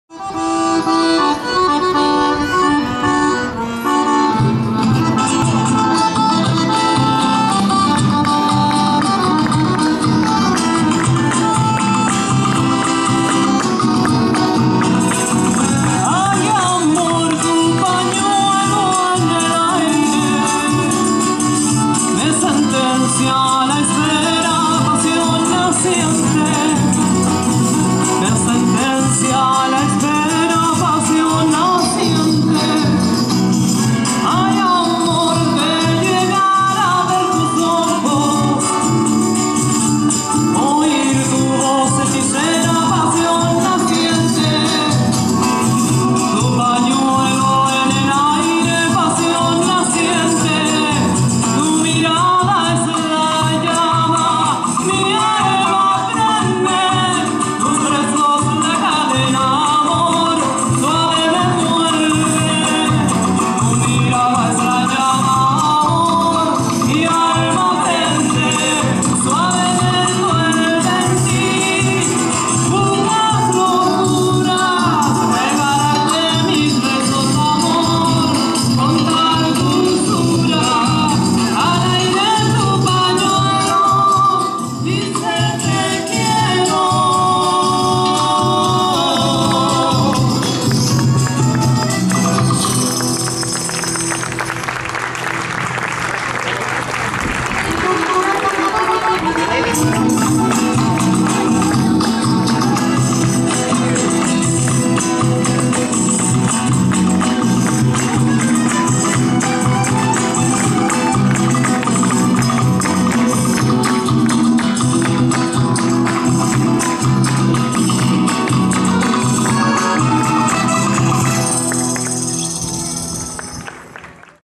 Ballet Folclorico de Coronel - Cuadro Huaso Chillan 2017
Ballet Folclórico de Coronel BAFCOR CHILE en el Festival de Danzas Folclóricas realizado en Chillán los días 24, 25 y 26 de Noviembre de 2017.